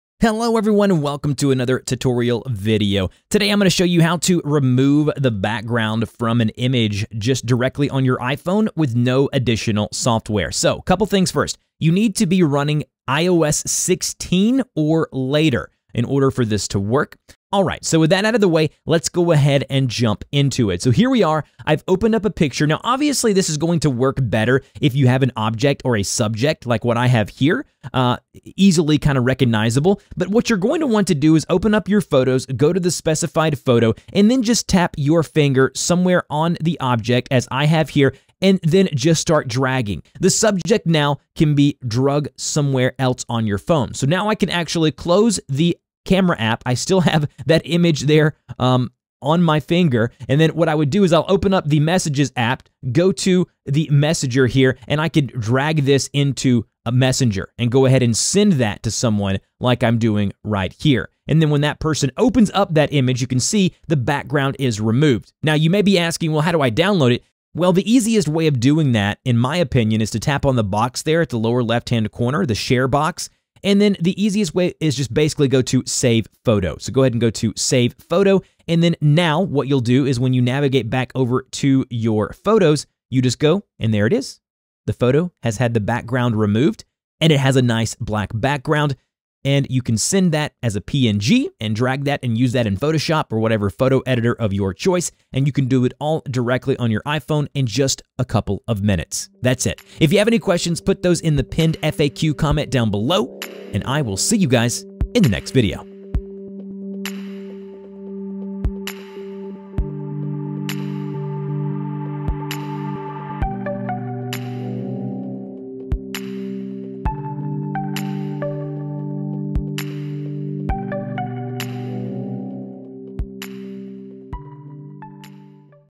How To Remove Background From A Photo On iPhone - NO Software Required
In this video tutorial, I show you how to remove a background from a photo on your iPhone using NO additional software.

MUST be using iOS 16 or LATER.